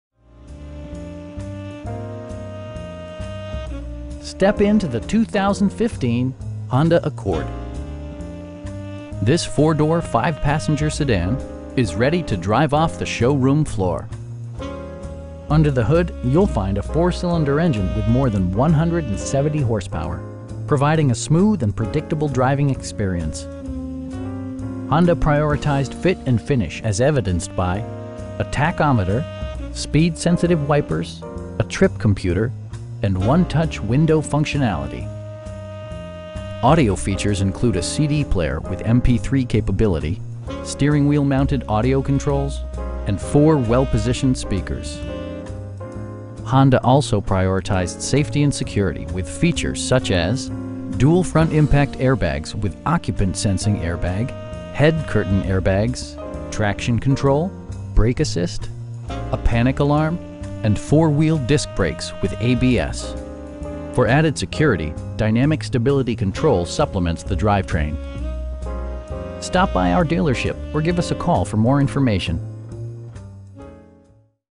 2015 Honda Accord Sport in Kingwood, TX 77339
Sterling McCall Honda
22575 Highway 59 N

Step into the 2015 Honda Accord. This 4 door, 5 passenger sedan is ready to drive off the showroom floor! Under the hood you'll find a 4 cylinder engine with more than 170 horsepower, providing a smooth and predictable driving experience. Honda prioritized fit and finish as evidenced by: a tachometer, speed sensitive wipers, a trip computer, front bucket seats, power door mirrors, power windows, and 1-touch window functionality. Audio features include a CD player with MP3 capability, steering wheel mounted audio controls, and 4 speakers, providing excellent sound throughout the cabin. Honda also prioritized safety and security with features such as: dual front impact airbags with occupant sensing airbag, head curtain airbags, traction control, brake assist, a panic alarm, and 4 wheel disc brakes with ABS. For added security, dynamic Stability Control supplements the drivetrain. You will have a pleasant shopping experience that is fun, informative, and never high pressured. Stop by our dealership or give us a call for more information.